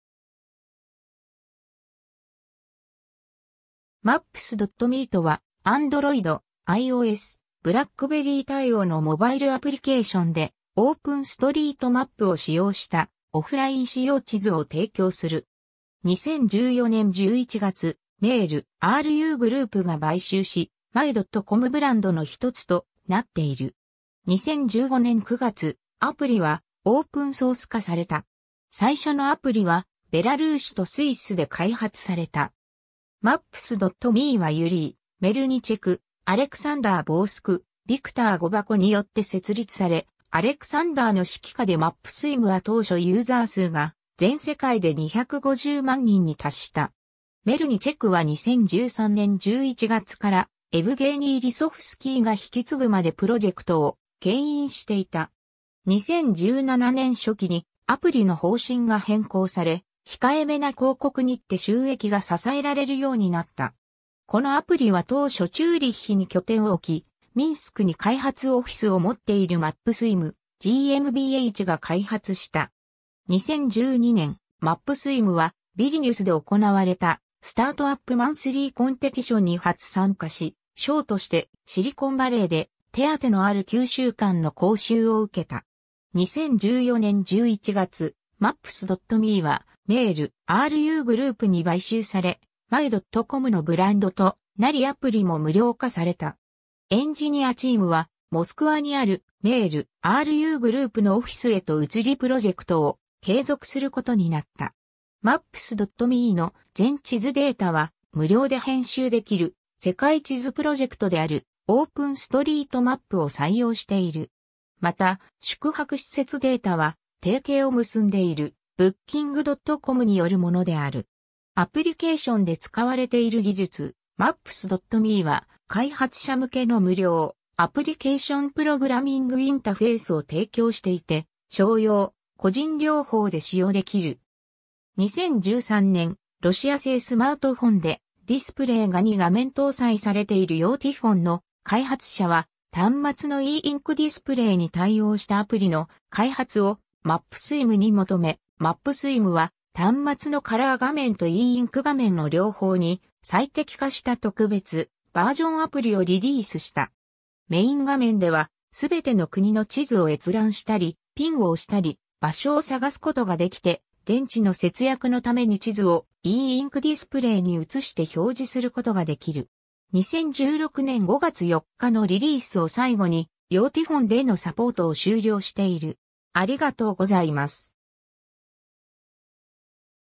Maps.me
Maps.me(MAPS.MEとも表記、過去名にMapsWithMe)とはAndroid、iOS、BlackBerry対応のモバイルアプリケーションで、オープンストリートマップを使用したオフライン使用地図を提供する。
2014年11月、Mail.Ru Groupが買収し、My.comブランドの1つとなっている。
2015年9月、アプリはオープンソース化された。
最初のアプリはベラルーシとスイスで開発された。
Maps.meはユリー・メルニチェク(Yury Melnichek)、アレクサンダー・ボースク(Alexander Borsuk)、ヴィクター・ゴヴァコ(Viktor Govako)によって設立され、アレクサンダーの指揮下でMapsWithMeは当初ユーザー数が全世界で250万人に達した。
メルニチェクは2013年11月からエヴゲーニー・リソフスキー(Evgeny Lisovskiy)が引き継ぐまでプロジェクトを牽引していた。
2017年初期にアプリの方針が変更され、控えめな広告にって収益が支えられるようになった。
 このアプリは当初チューリッヒに拠点を置き、ミンスクに開発オフィスを持っているMapsWithMe GmbHが開発した。
 2012年、MapsWithMeはヴィリニュスで行われたスタートアップ・マンスリーコンペティションに初参加し、賞としてシリコンバレーで手当のある9週間の講習を受けた。
 2014年11月、Maps.meはMail.Ru Groupに買収され、My.comのブランドとなりアプリも無料化された。
エンジニアチームはモスクワにあるMail.Ru Groupのオフィスへと移りプロジェクトを継続することになった。
 Maps.meの全地図データは無料で編集できる世界地図プロジェクトであるオープンストリートマップを採用している。
また、宿泊施設データは提携を結んでいるブッキングドットコムによるものである。
 アプリケーションで使われている技術: Maps.meは開発者向けの無料アプリケーションプログラミングインタフェースを提供していて商用・個人両方で使用できる。
 2013年、ロシア製スマートフォンでディスプレイが2画面（前面LCD、背面eインク）搭載されているYotaPhoneの開発者は端末のeインクディスプレイに対応したアプリの開発をMapsWithMeに求め、MapsWithMeは端末のカラー画面とeインク画面の両方に最適化した特別バージョンアプリをリリースした。
 メイン画面では全ての国の地図を閲覧したり、ピンを押したり、場所を探すことができて、電池の節約のために地図をeインクディスプレイに移して表示することができる。
 2016年5月4日のリリースを最後にYotaPhoneでのサポートを終了している。